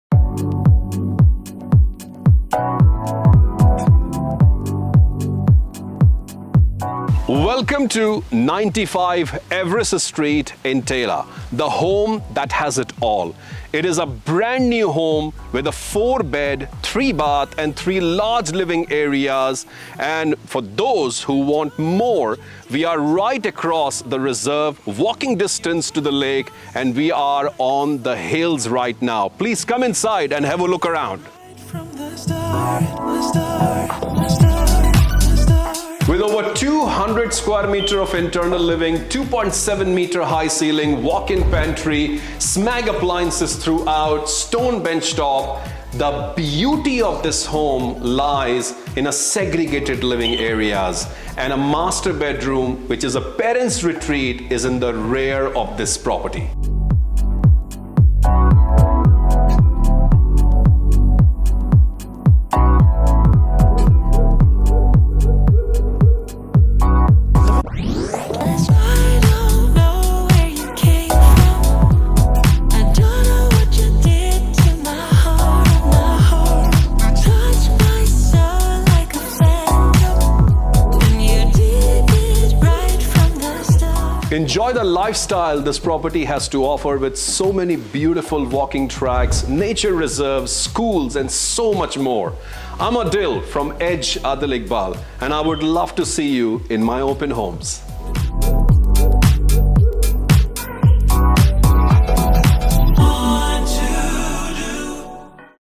95 Everist Street Taylor ACT - Absolutely gorgeous!
A sublime north facing home nestled in the most sought after suburb of Taylor, this incredible 4 bedroom and three bathroom home is a true exemplar of modern living and design. With impressive street appeal and engineered for comfort, this family home offers a high standard of living. Features of this 587m2 parcel of land include hybrid flooring, 2.7m2 high ceilings, chic built-ins, ducted reverse heating and cooking, sun-drenched rooms, manicured backyard with veggie garden and custom floor plan.

Beyond a tasteful hallway adorned with a large lounge room for family and friends' reunion, the main floor of the home contains a massive family and dining room with an open-concept kitchen. The stylish kitchen is equipped with a 20mm Caesar stone benchtop, marble countertops, a matching backsplash, custom cabinets, high polish joinery, soft close drawers, a large walk-in pantry and a suite of high-end appliances from Smeg including 900mm oven and induction cooktop. The family room opens to a large rumpus room overlooking the backyard and has private access to the deck. The rumpus could be used as multi-purpose room, a home office or kids' retreat.

The master suite boasts a beautiful walk-in closet, a windowed en-suite bathroom with floor to ceiling tiles, dual vanities, a walk-in rain shower and matt black taps. The other two bedrooms in the rear have large built-in robes and have easy access to a full bathroom with bathtub. The bedroom on the front has its own bathroom and could be used as a guest room. It has double glazing to widows and sliding doors which mean the high 7.4 energy rating makes it cheaper to heat and cool than a similar sized lesser rating home.

The home is near nature with lots of green pastures and walking tracks where one can catch morning and evening sun while going for a sprint or walking a dog. The home is just a short distance away from trendy restaurants, cafes, shops, and is close to great schools. This home is also close to Gungahlin market place, Belconnen mall, and few mins drive to the City.

Key Features;
- In front of reserve and walking distance to lake
- Bathrooms with floor to ceiling tiles
- Wall mounted vanities and semi frameless shower screens
- Kitchen with 20mm Caesar stone benchtop
- Splashback, induction cooktop and 900mm oven from Smeg
- Soft close drawers and high polish joinery
- Deck for outdoor entertainment with fantastic views
- 3 separate living areas including formal lounge, family/dining and rumpus room all with hybrid flooring
- Large garage with roller door to carport and internal access
- Natural grass in the front and backyard with veggie patch and lots of room for kids' play equipment
- 2.7m high ceiling with square set cornices
- Large walk-in pantry with lots of cupboard spaces
- Instant hot water system
- 2000L rain water
- Double glazing to windows and doors
- Walking distance to a lake, bus stop and schools